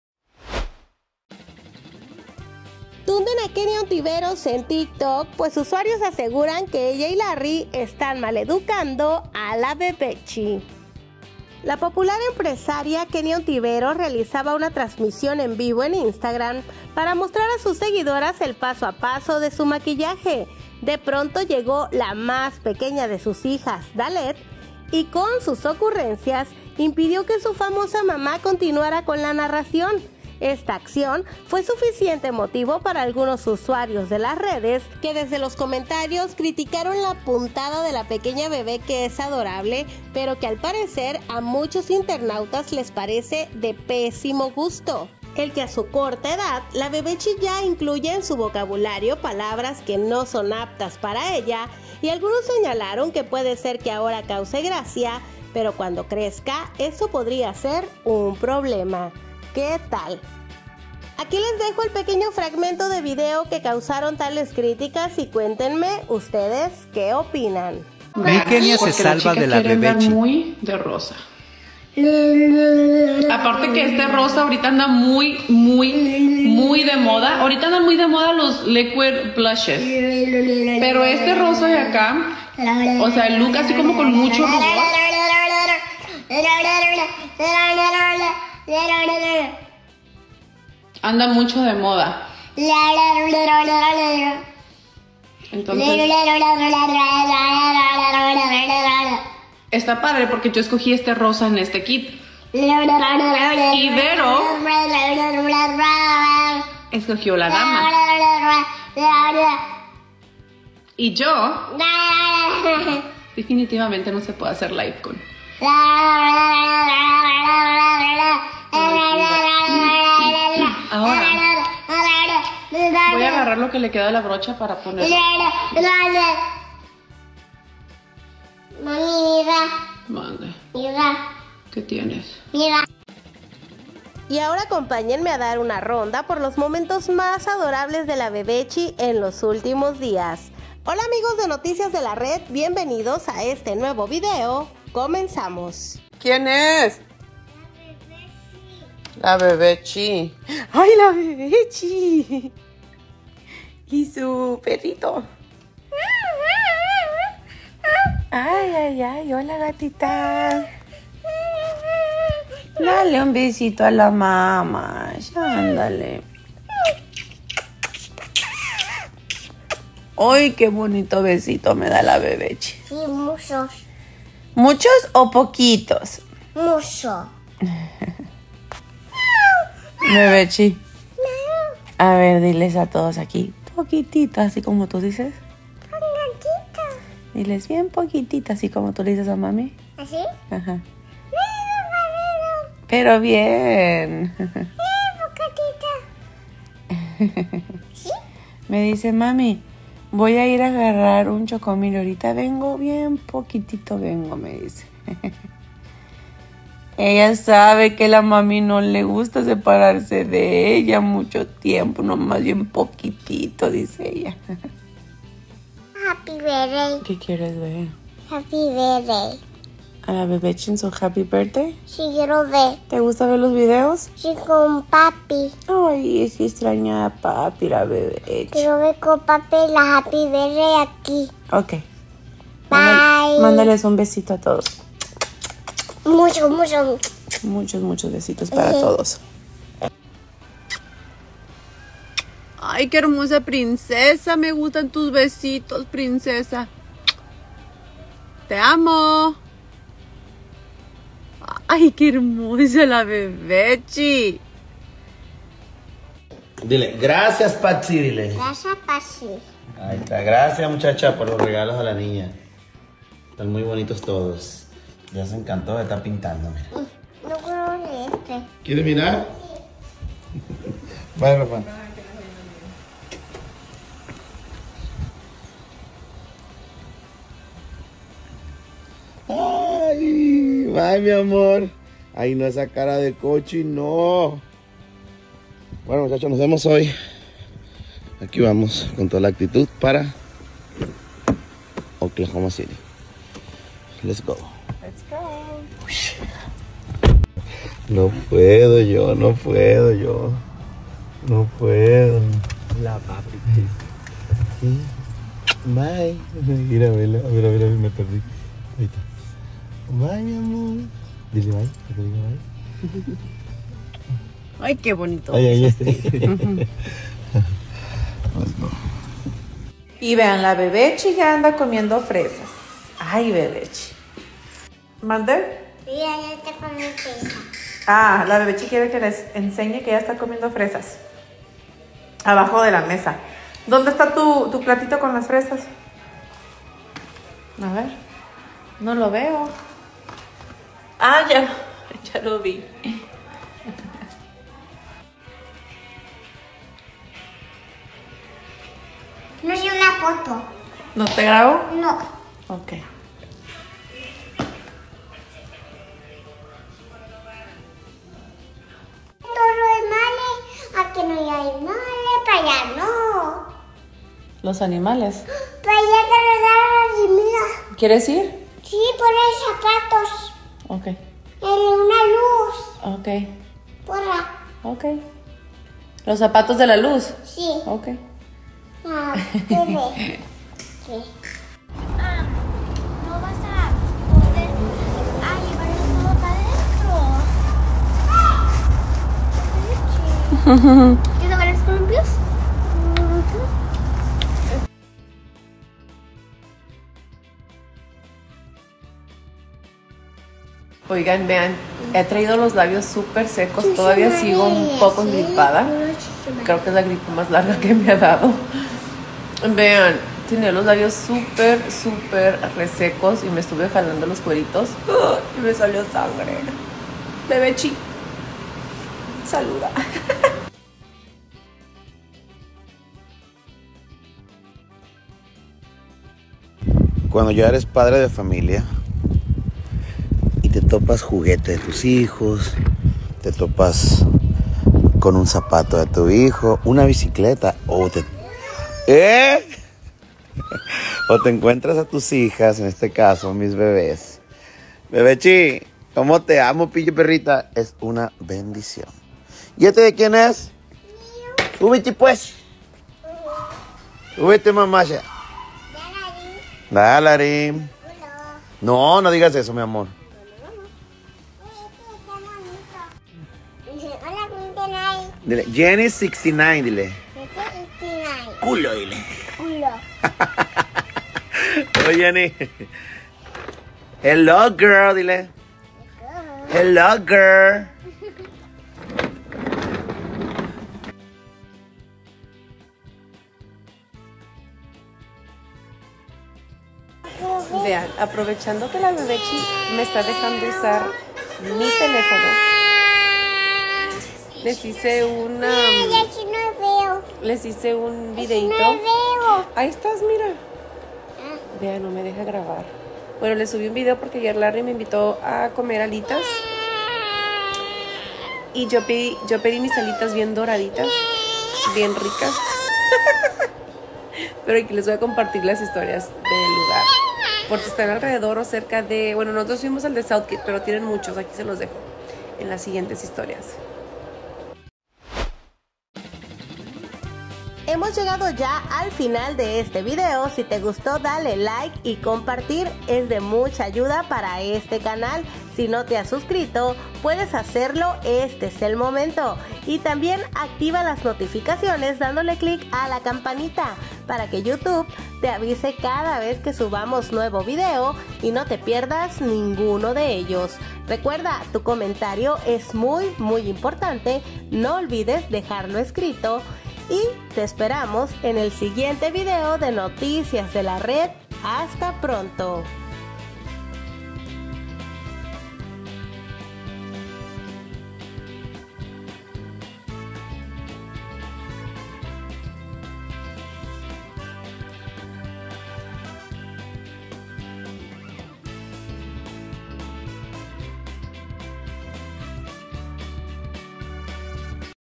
CRITICAN A KENIA ONTIVEROS POR WL COMPORTAMIENTO DE LA BEBECHI. NOTICIAS DE LA RED .
LA BEBECHI INTERRUMPE A KENIA EN UNA TRANSMISIÓN EN VIVO Y LA CRITICAN.